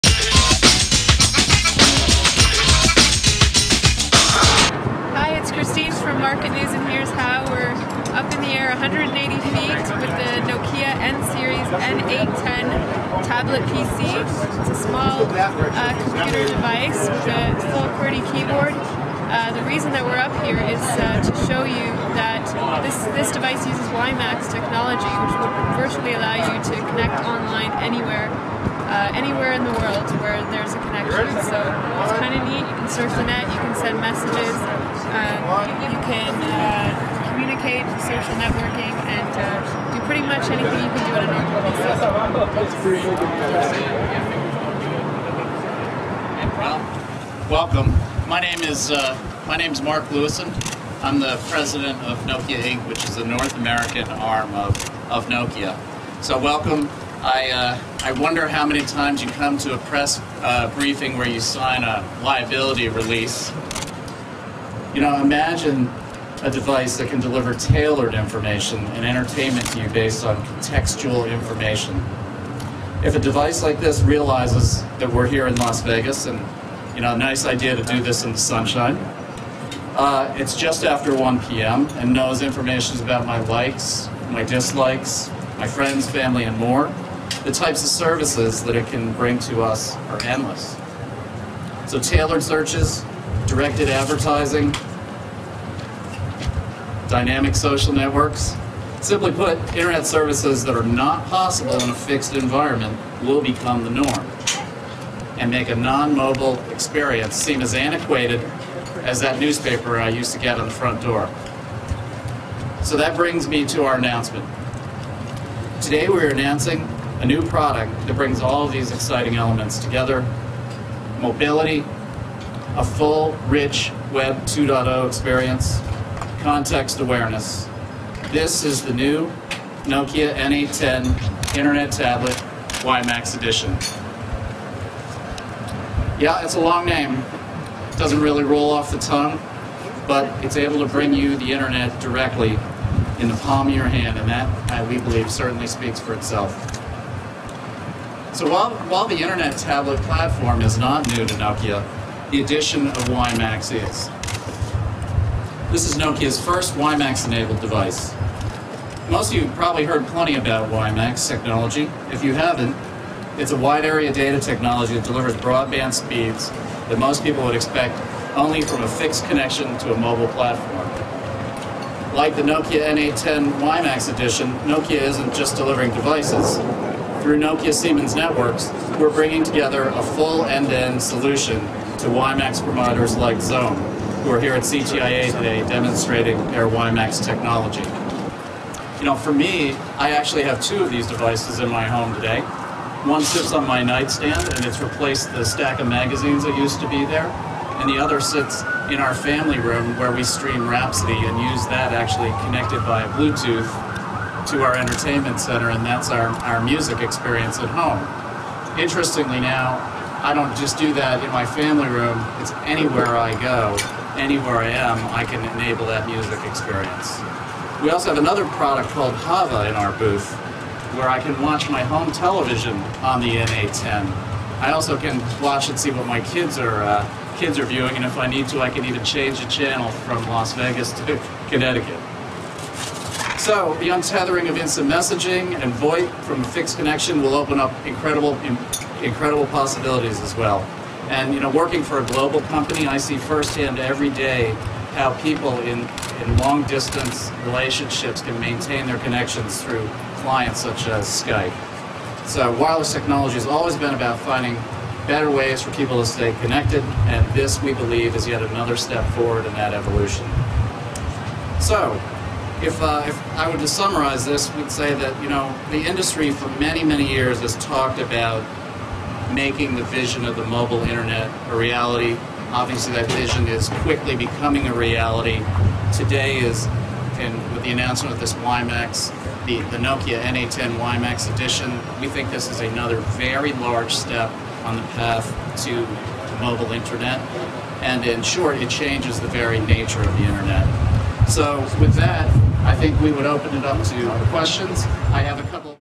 Flying High with Nokia and WiMax
Christine Persaud of Marketnews speaks to us from 180 ft, in the air at a Nokia press conference for the N series N810 Tablet PC. Nokia demonstrates with WiMax technology incorporated in this new device one has the ability to connect online any where in the world.